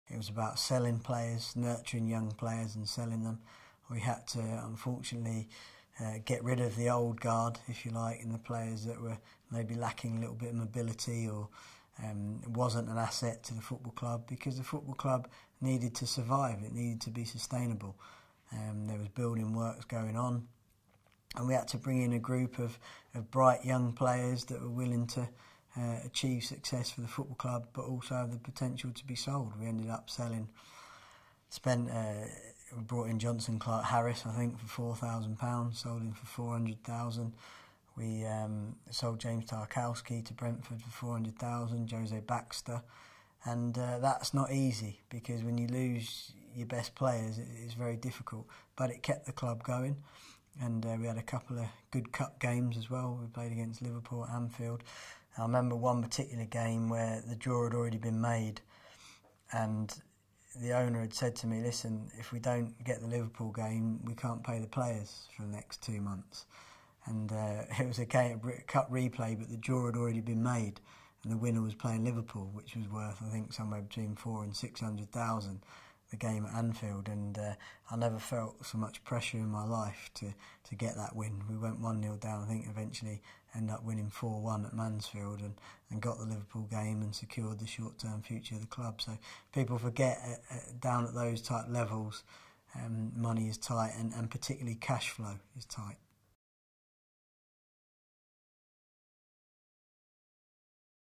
Lee Johnson • "We had to win an FA Cup tie to pay the players' wages" • CV Stories
Lee Johnson reflects on his time as Oldham Athletic manager, discussing the challenges of losing his best players and putting faith in the club's academy to replace those outgoing.